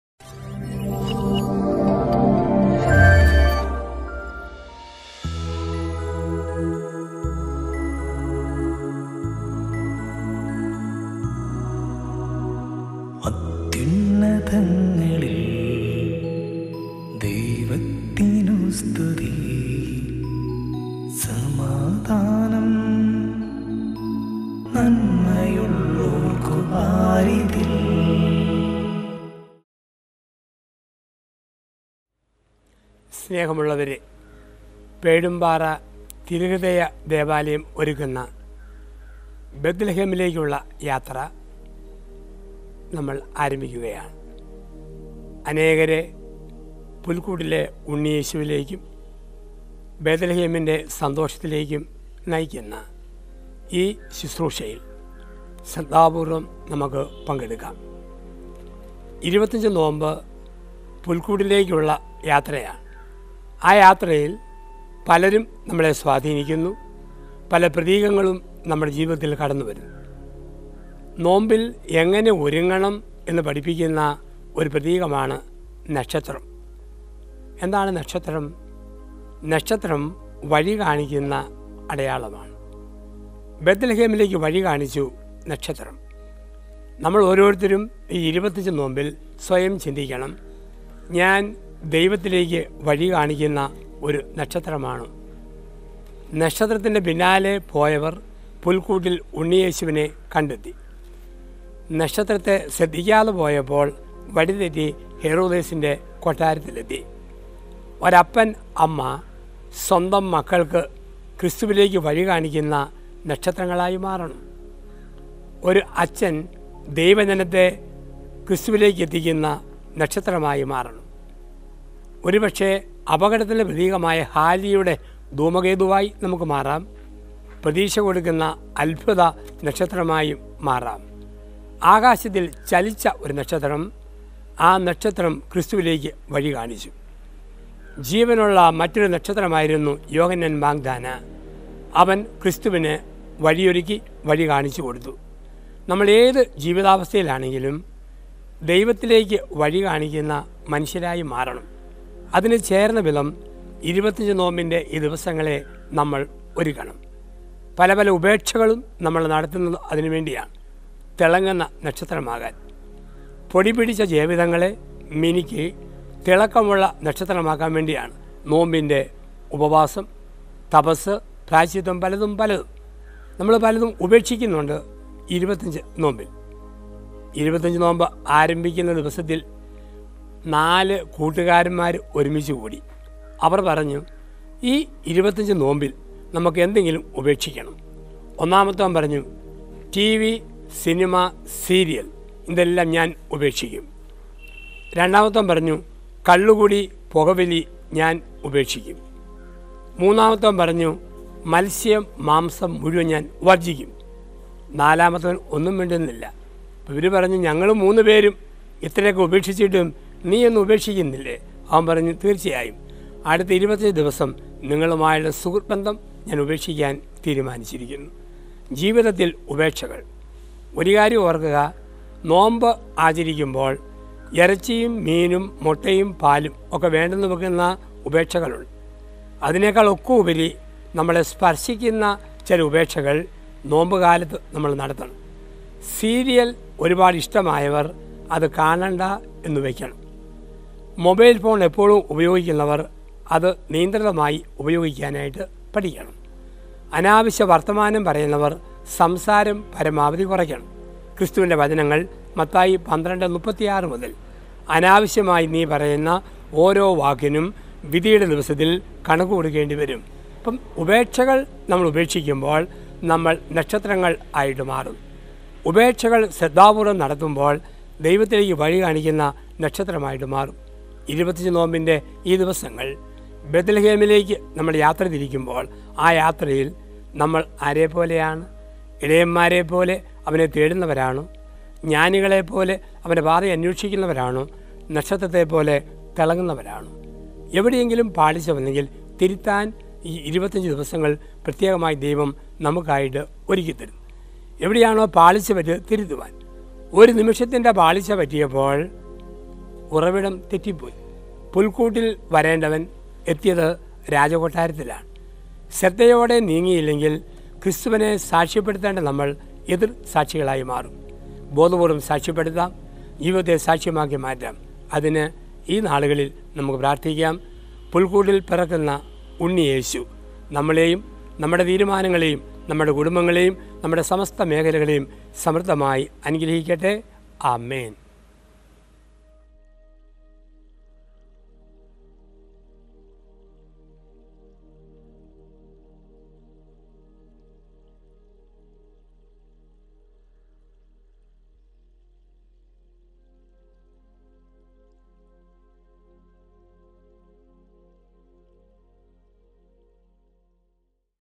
The Journey to Bethlehem Dec.2 | Message by Fr.Joseph Puthenpurackal |ബെത്‌ലഹേമിലേക്കുള്ള യാത്ര
ബേത്‌ലഹേമിലേക്കുള്ള യാത്ര
25 നോമ്പ് എങ്ങനെ ഫലപ്രദമായി ആചരിക്കാം എന്നതിനെക്കുറിച്ച്
രണ്ടാം ദിവസം
ബഹുമാനപ്പെട്ട  പുത്തൻപുരയ്‌ക്കൽ അച്ചന്റെ 25 നോമ്പ്  സന്ദേശം